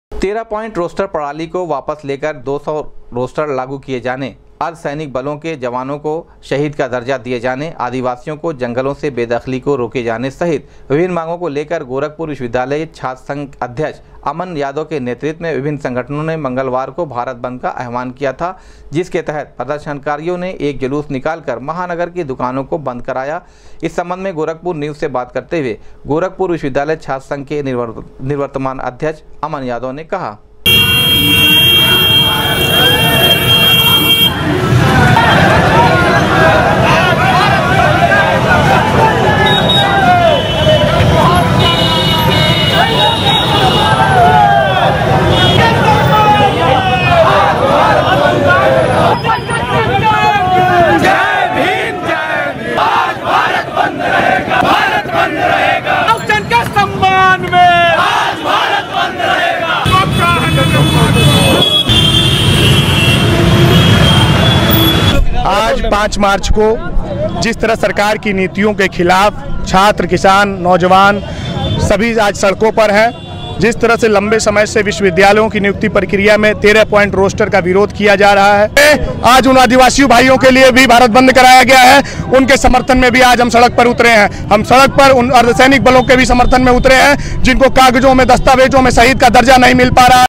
13 प्वाइंट रोस्टर प्रणाली को वापस लिए जाने सहित विभिन्न मांगों को लेकर छात्र संगठनों
13 प्वाइंट रोस्टर प्रणाली को वापस लिए जाने सहित विभिन्न मांगों को लेकर छात्र संगठनों ने किया भारत बंद का आहवान, मांगे पूरी नही होने पर दी उग्र आंदोलन की चेतावनी